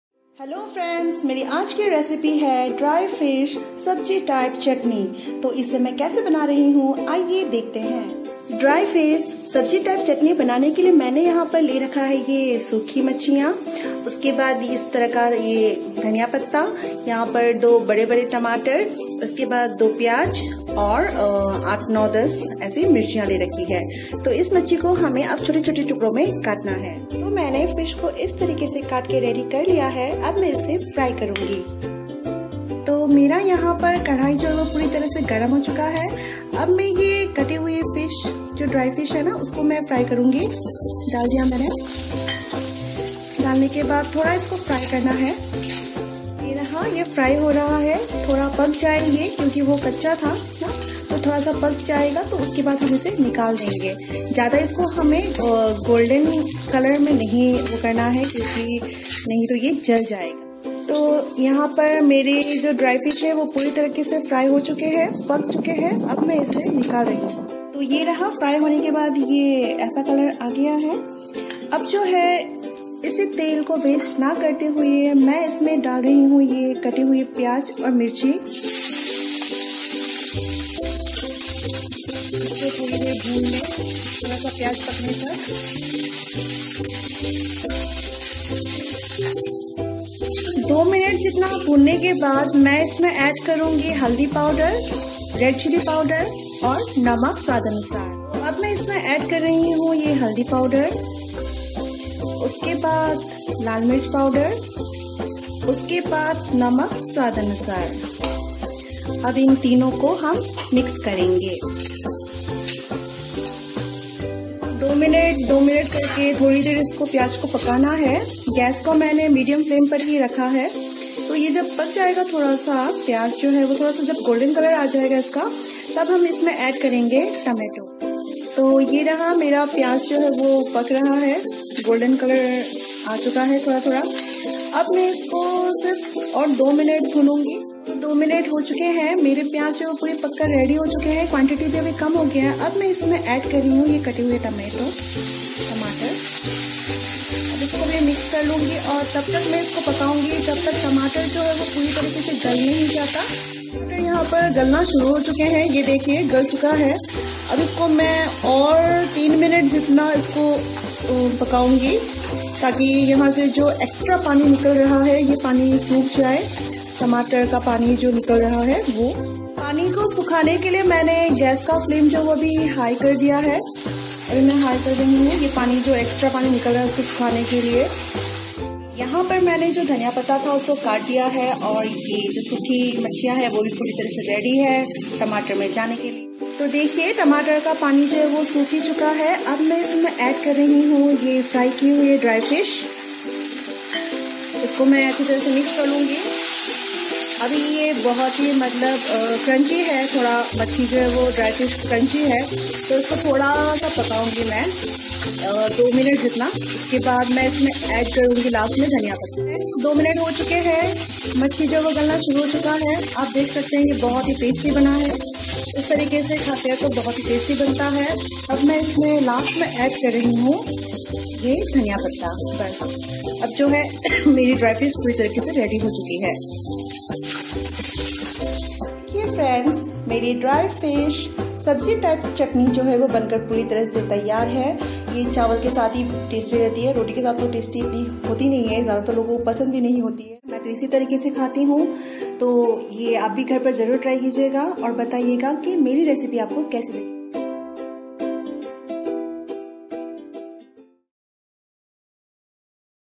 Tasty tasty dry fish sabji type chutney ll सूखी मछलियों की सब्जी टाइप टेस्टी चटनी ll
Hlw friends,
Aaj ki easy recipe hai dry fish sabji type Chutney. Ye banane mei easy aur khane mei bahut hi tasty lagti. Hope u all like it.